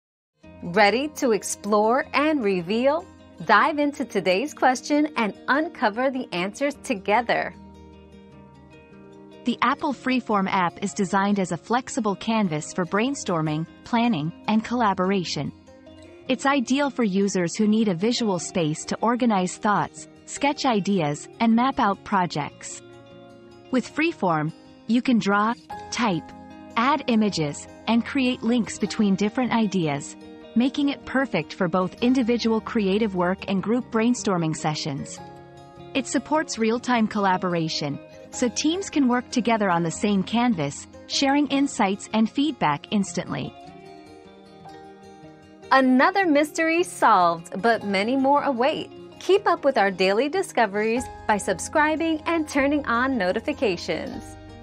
What is Apple freeform app used for?
Unleash Your Creativity with Apple Freeform 👉 Unleash Your Creativity 👉 Discover the power of Apple Freeform, the ultimate brainstorming and collaboration tool. With its flexible canvas, you can organize thoughts, sketch ideas, and map out projects. Whether you're working individually or in a team, Freeform allows you to draw, type, add images, and create links between ideas. Experience real-time collaboration and unleash your creativity with Apple Freeform!

Laura S. Harris (2024, January 29.) What is Apple freeform app used for?
    🌐 AskAbout.video